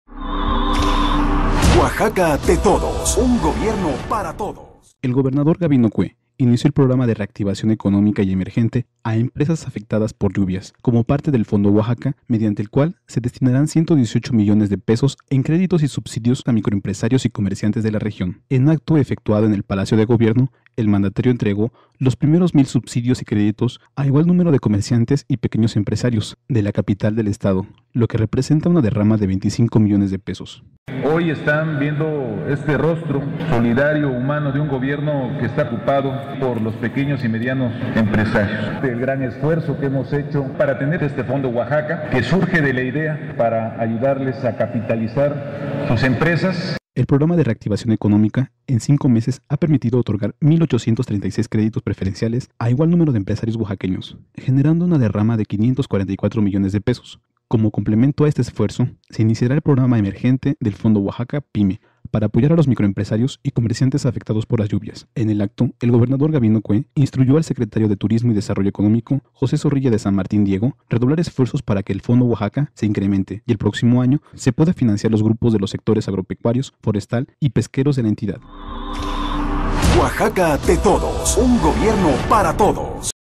Inicia Gabino Cué la entrega de 118 mdp a microempresarios y comerciantes afectados por lluvias
El gobernador Gabino Cué Monteagudo puso en marcha el Programa de Reactivación Económica y Emergente a Empresas Afectadas por las Lluvias, que se enmarca dentro del Fondo Oaxaca, mediante el cual se destinarán 118 millones de pesos en créditos y subsidios para seis mil  micro empresarios y comerciantes de los Valles Centrales y otras regiones de la entidad.

En  acto efectuado en el patio central de Palacio de Gobierno, y en presencia del coordinador del Fondo Emergente PyME por desastres naturales de la Secretaría de Economía del Gobierno Federal, Felipe Chávez Silva, el mandatario entregó de manera simbólica los primeros mil subsidios y créditos a igual número de comerciantes y pequeños empresarios de la capital del estado, lo que representa una derrama de 25 millones de pesos.

El Ejecutivo estatal recordó que en el pasado mes de julio se instituyó el Fondo Oaxaca  y con el apoyo de la Secretaría de Economía del Gobierno Federal, de Nacional Financiera, así como diversas instituciones de la Banca comercial y de desarrollo del país se integró una bolsa de financiamiento preferencial por mil millones de pesos, para capitalizar y brindar asistencia técnica a comerciantes, así como a las pequeñas y medianas empresas de la entidad.

Con estos recursos se puso en marcha el Programa de Reactivación Económica que en tan sólo cinco meses ha permitido otorgar mil 836 créditos preferenciales a igual número de emprendedores oaxaqueños, generando una derrama de 544 millones de pesos.

Ahora, como complemento a este esfuerzo se inicia el programa emergente del Fondo PyME para apoyar a microempresarios  y comerciantes afectados por las lluvias, con lo cual se eleva a dos mil 836 el número de beneficiados con este esquema.

En este programa emergente el 50.8  por ciento de los 118 millones de pesos asignados se destinarán como subsidios a fondo perdido hasta por un monto de 15 mil pesos a los pequeños empresarios y comerciantes que reunan los requisitos que demanda este  esquema.

El 49.2 por ciento restante de los recursos será canalizado a través de créditos por un rango de 15 mil a 150 mil pesos, con una tasa preferencial fija a tres años.

En  el marco de este acto, el gobernante estatal instruyó al Secretario de Turismo y Desarrollo Económico, José Zorrilla de San Martín Diego, redoblar esfuerzos para que el Fondo Oaxaca se incremente y el próximo año se pueda financiar a grupos de los sectores agropecuario, forestal y pesquero de la entidad.

En su oportunidad, el secretario de Turismo y Desarrollo Económico indicó que en los próximos días se entregarán estos mismos apoyos en la ciudad puerto de Salina Cruz, con lo  cual se estará atendiendo a un total de mil 500 empresarios en esta primera fase del programa emergente, cifra que muestra el incremento habido en comparación con las 437 micro y pequeñas empresas que en promedio se beneficiaban en años anteriores.

Destacó que es un orgullo que el fondo Oaxaca sea un estandarte de Nacional Financiera y a su vez un referente para promover la creación  de instancias similares en otros estados, al tiempo que exhortó a los microempresarios y comerciantes a aprovechar las acciones de orientación y capacitación que en materia financiera les ofrece el fondo.

De su lado el  director estatal de Nacional Financiera, Jesús Ruiz Pérez, manifestó estas acciones son resultado de un trabajo en equipo y refrendó el apoyo de ese organismo a todas las acciones que emprenda el gobierno de Gabino Cué  para impulsar el desarrollo y crecimiento del sector empresarial.

A nombre de los beneficiados intervino Matilde Martínez Blas quien agradeció  que el gobierno estatal y federal se preocupe y  proporcione los apoyos para resarcir las pérdidas patrimoniales de los pequeños empresarios y comerciantes que han resentido los efectos de las catástrofes climáticas, como se demuestra con la entrega de estos créditos y subsidios.

Oaxaca de Juárez, 20 de diciembre del 2011